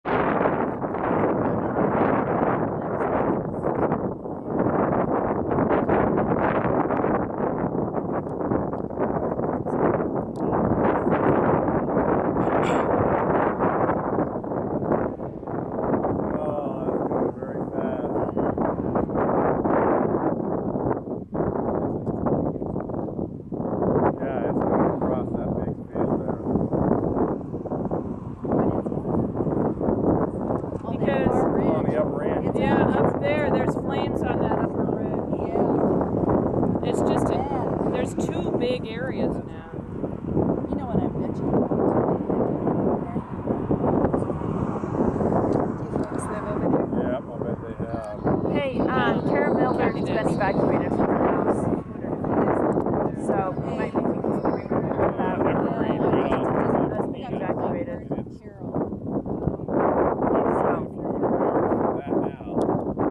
Pocatello, ID. Mink Creek area fire: June 28, 2012
Brief video of the fire which destroyed many houses in the Mind Creek area near Pocatello, ID on June 28, 2012 05:36PM.
The plume of black smoke near the flames followed an explosion which was probably a propane tank, which is common in the area.